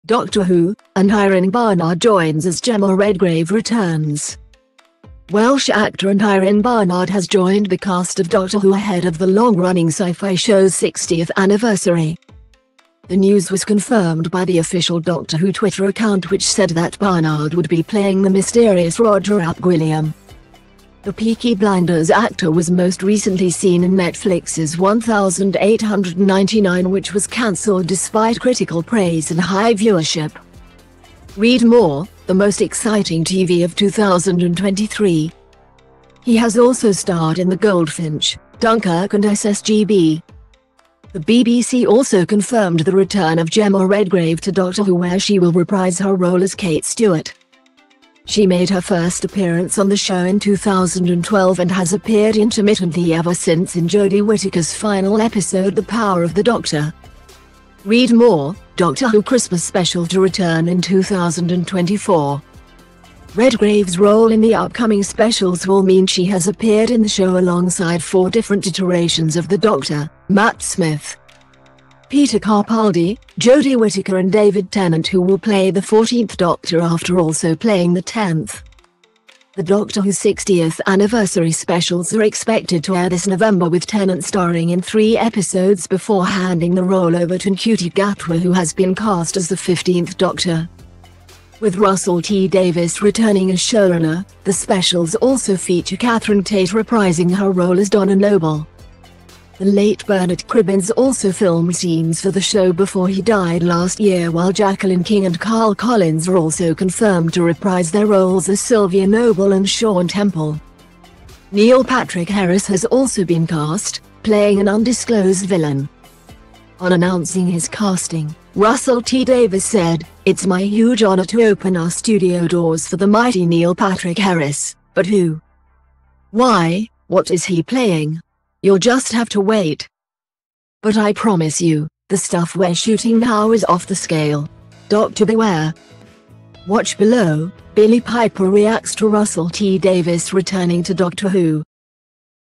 Doctor Who: Aneurin Barnard joins as Jemma Redgrave returns
Welsh actor Aneurin Barnard has joined the cast of Doctor Who ahead of the long-running sci-fi show's 60th anniversary. The news was confirmed by the official Doctor Who Twitter account which said that Barnard would be playing "the mysterious Roger ap Gwilliam". The Peaky Blinders actor was most recently seen in Netflix's 1899 which was cancelled despite critical praise and high viewership.